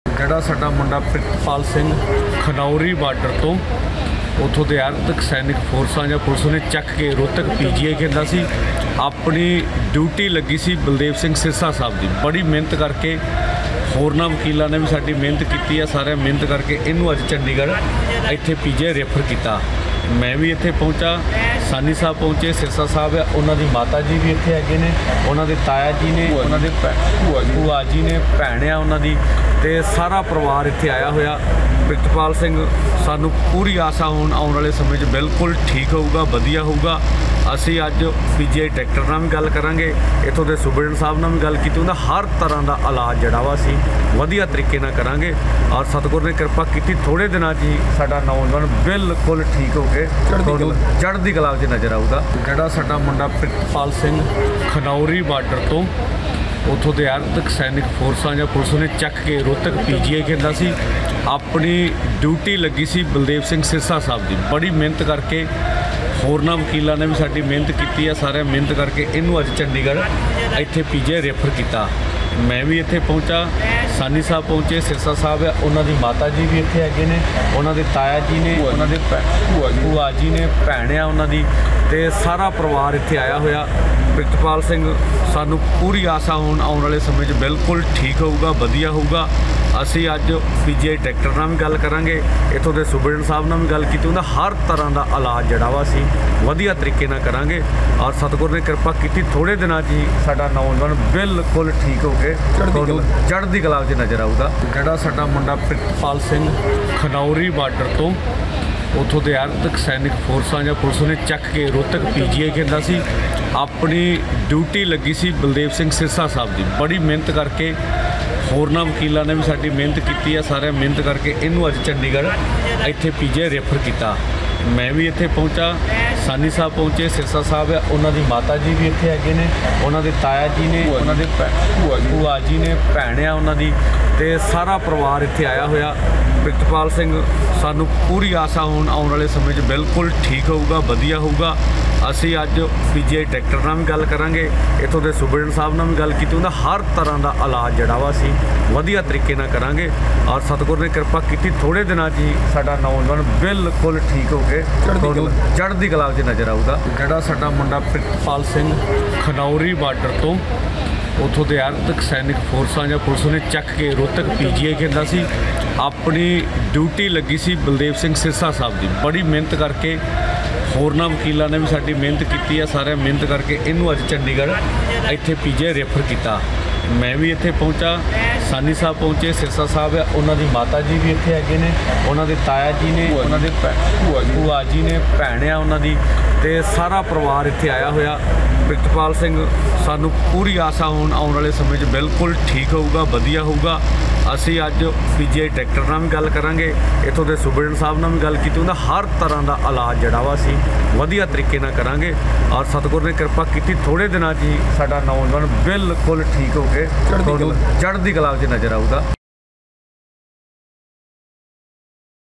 Farmer Protest: ਜਖ਼ਮੀ ਪ੍ਰਿਤਪਾਲ ਦਾ PGI ’ਚ ਇਲਾਜ, ਸਿਹਤ ’ਚ ਲਗਾਤਾਰ ਹੋ ਰਿਹਾ ਸੁਧਾਰ
ਜਖ਼ਮੀ ਪ੍ਰਿਤਪਾਲ ਦਾ PGI ’ਚ ਇਲਾਜ, ਸਿਹਤ ’ਚ ਲਗਾਤਾਰ ਹੋ ਰਿਹਾ ਸੁਧਾਰ